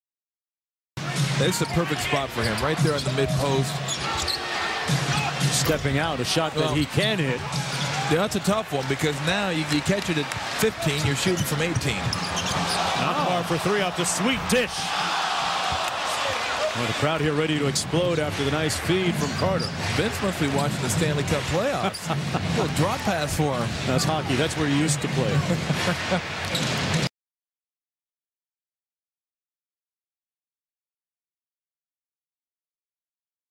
Vince Carter drops a sweet between-the-legs dish to Boki Nachbar, whose 3 goes halfway down & out
Friday, April 27, 2007. Game #3 of the 2007 Eastern Conference 1st Round between the New Jersey Nets & the Toronto Raptors at Continental Airlines Arena.